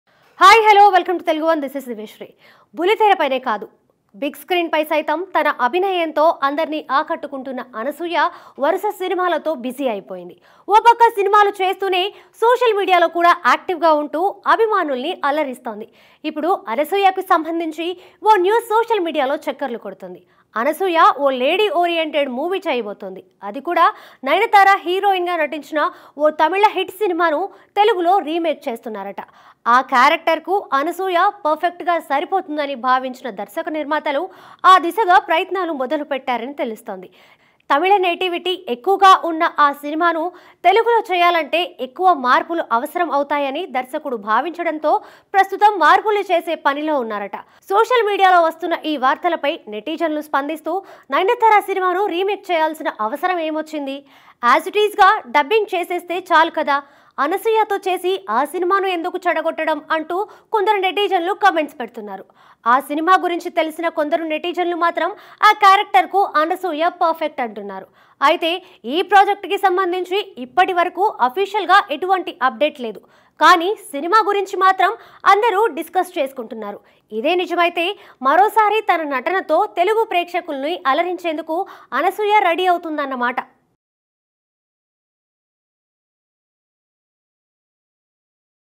నయనతార స్థానంలో అనసూయ..! Anasuya replaces Nayanthara..! TeluguOne
Anasuya Going To Remake A Nayanthara Movie. Anasuya Bharadwaj Latest movie updates.